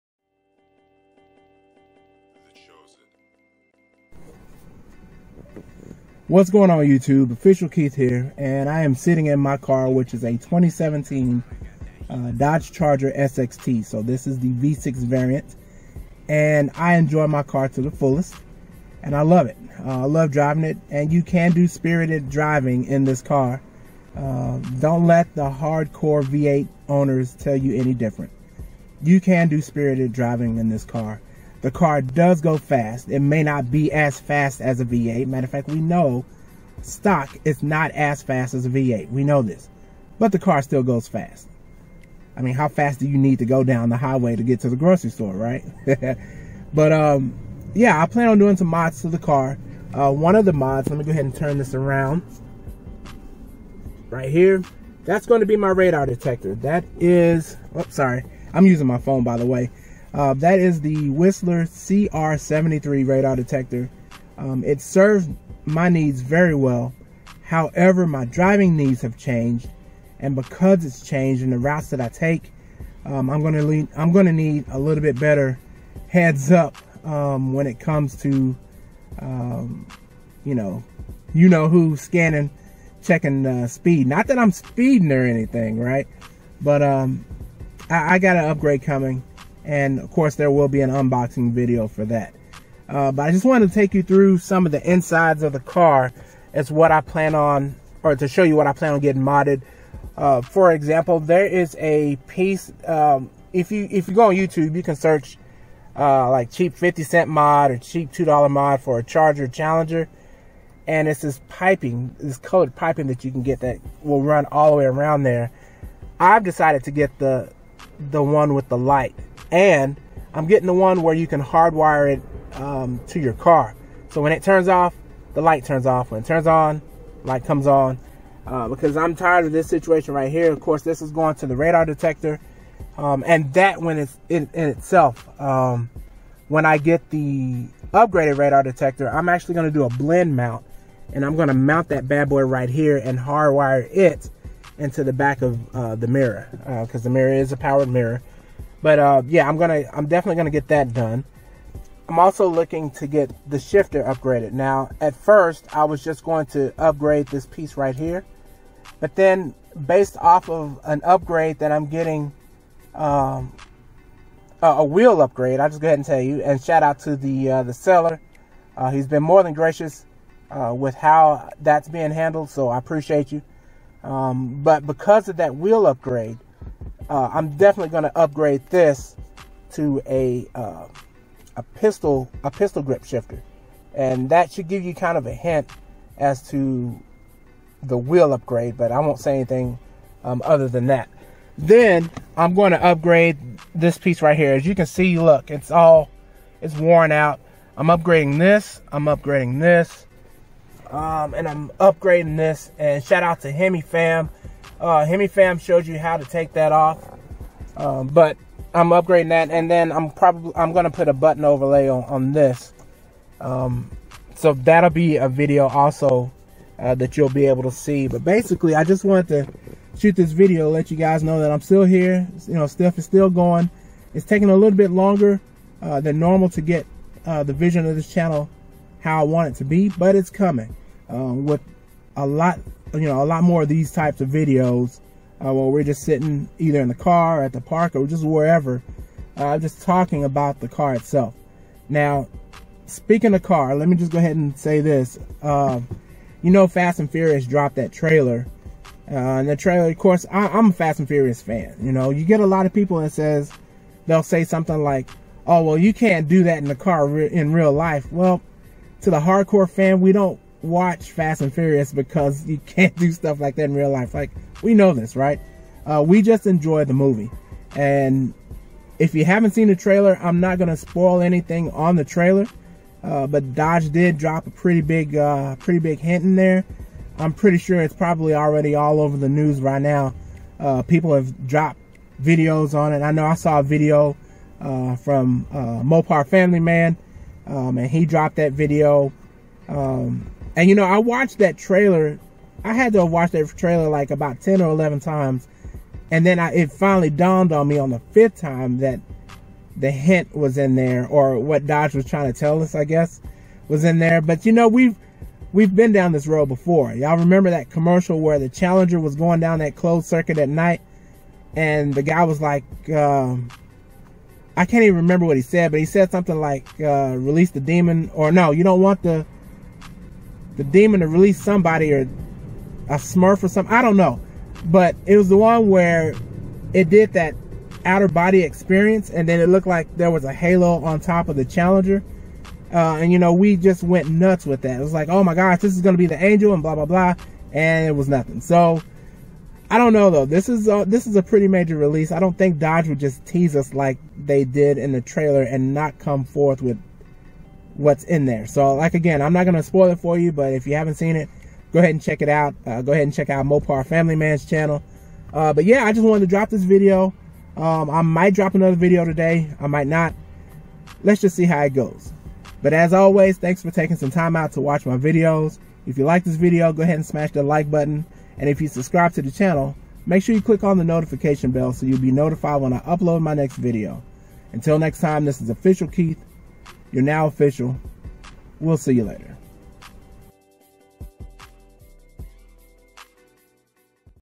Mods For The Dodge Charger
In this video, I tell you about the coming changes to Starscream my 2017 Dodge Charger SXT and to introduce more vlog style videos to the channel. Enjoy!